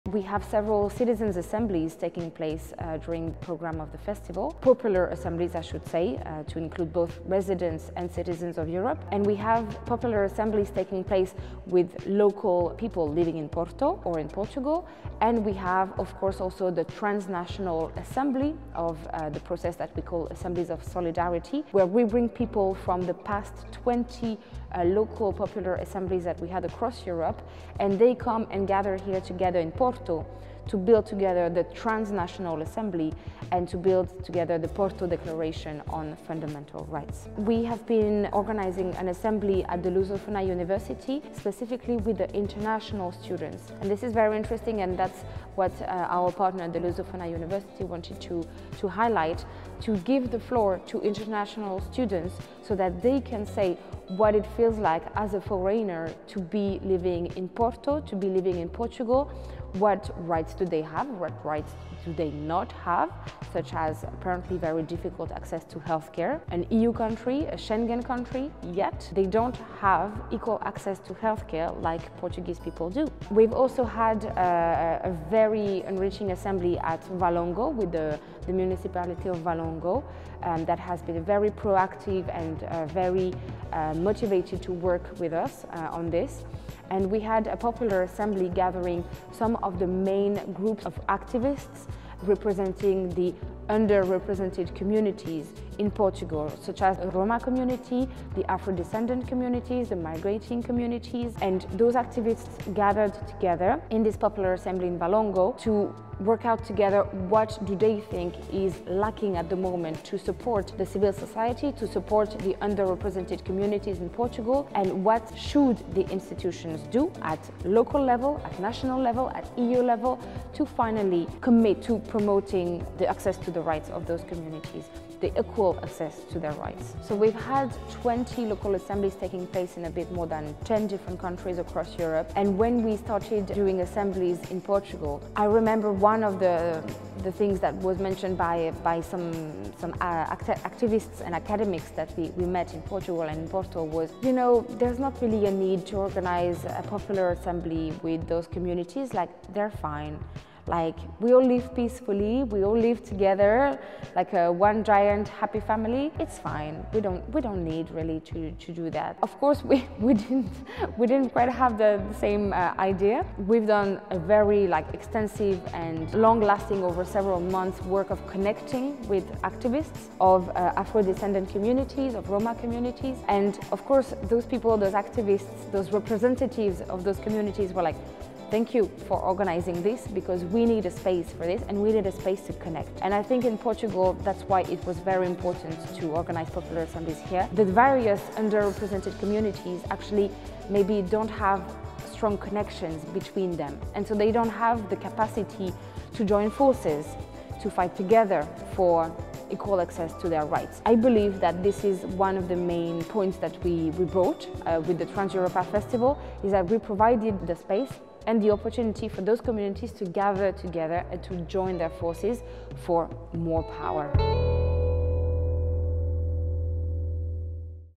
TRANSEUROPA 2022 hosted several popular assemblies!
“The various underrepresented communities actually don’t have strong connections between them and so they don’t have the capacity to join forces, to fight together for equal access to their rights. I believe that giving these people a common platform was one of the main achievements of the Transeuropa festival.”

One of the main aims of organizing popular assemblies in Porto was to include and listen to often underrepresented local voices, but also to create a network of activists capable of future collective actions! ✊🏾🔥

Listen to the full interview with Ophélie!